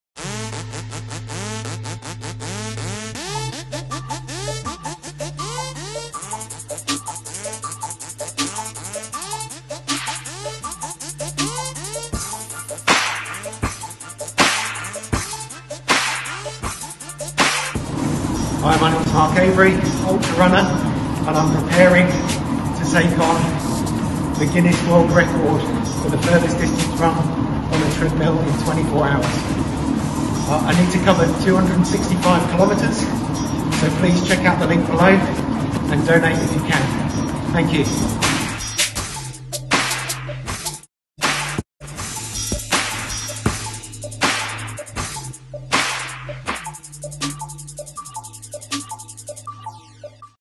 Mark Avery prepares to break the record for the furthest distance run on a treadmill in 24 hours.
Mark Avery preparing for his attempt to break the Guinness World Record for the furthest distance run on a treadmill in 24 hours to raise funds for The Cancer Council.
Global wellness leader Technogym is backing with Avery and the OUTRUN CANCER initiative in this fundraising.